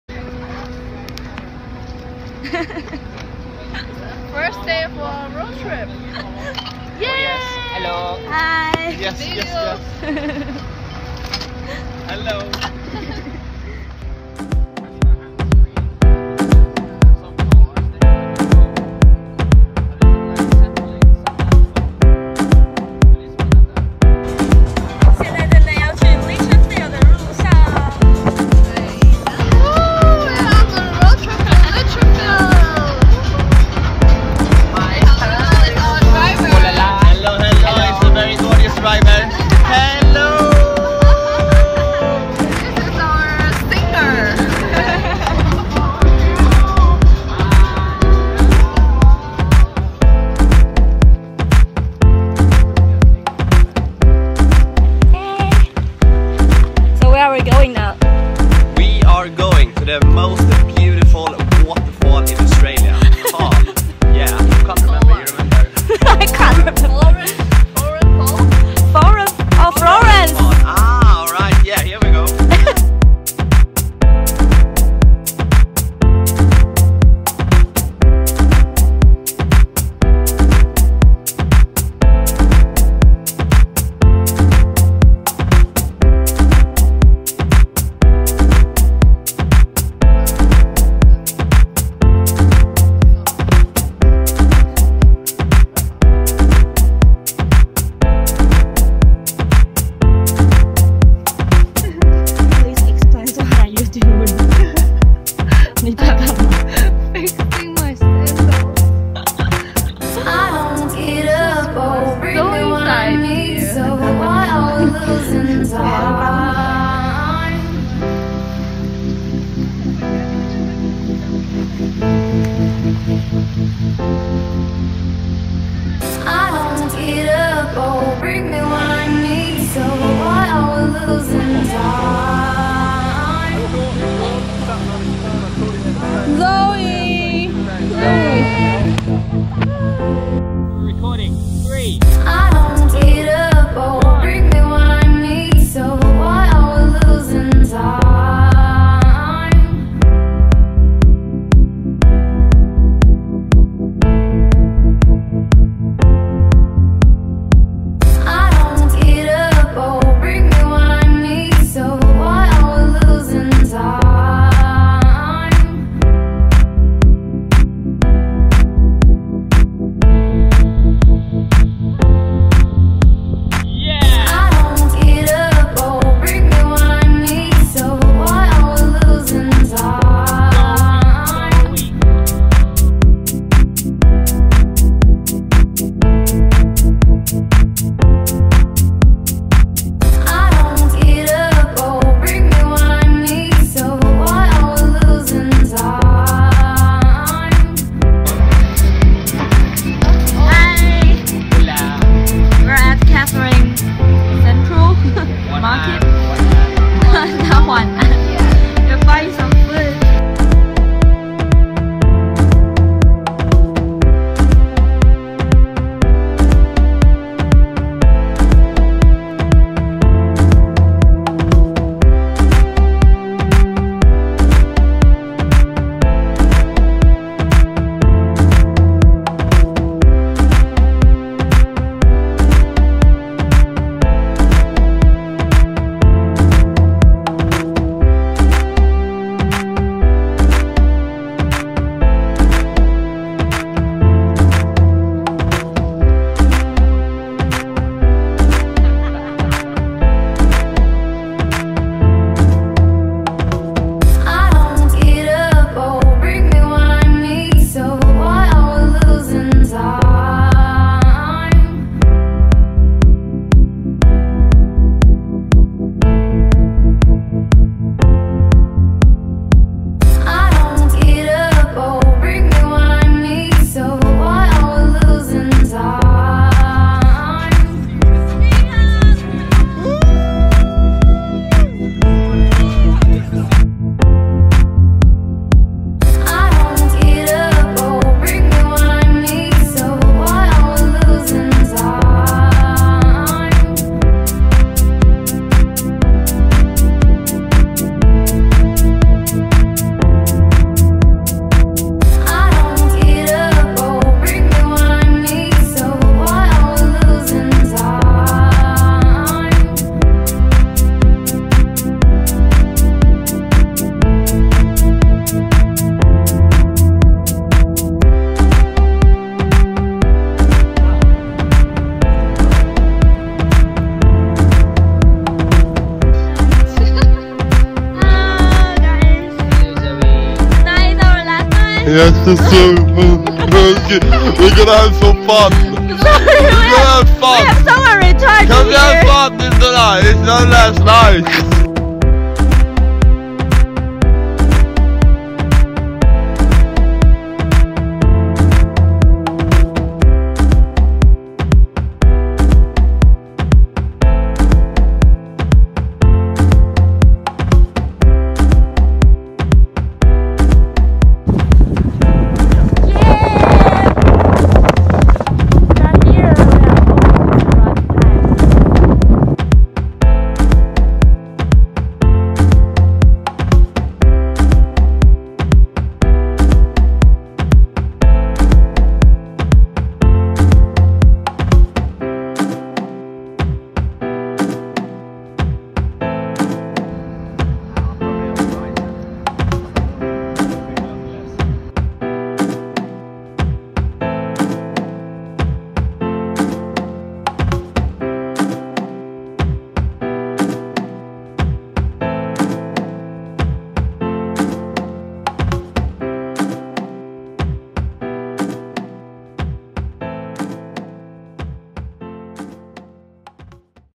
Road Trip in OZ : Darwin to Carins 達爾文-凱恩斯 公路旅行
澳洲公路旅行：從達爾文到凱恩斯/六天旅行
數度拼錯Carins（凱恩斯）請見諒。

Infos box-